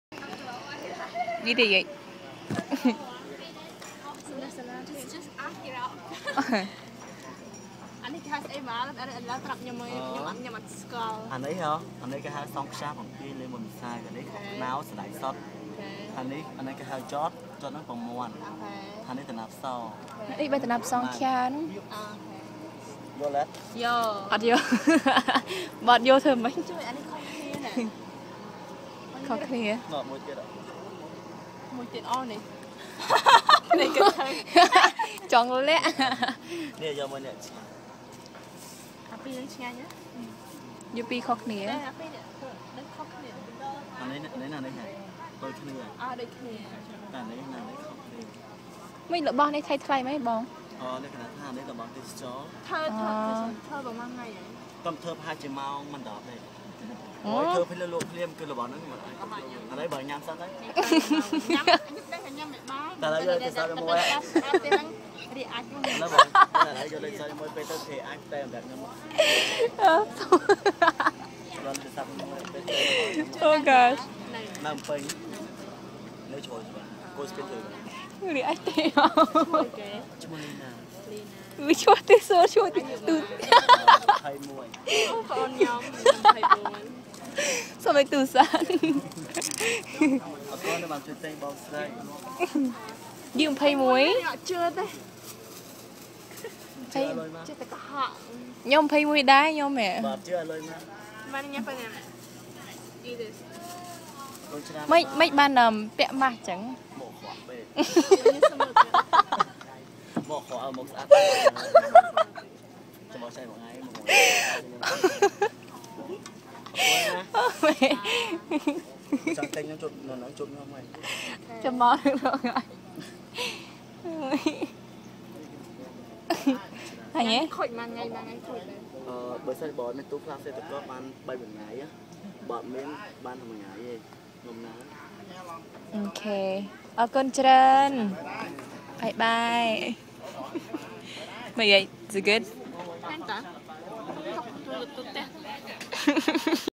Cambodia Trip
Just teasing around with this funny dessert seller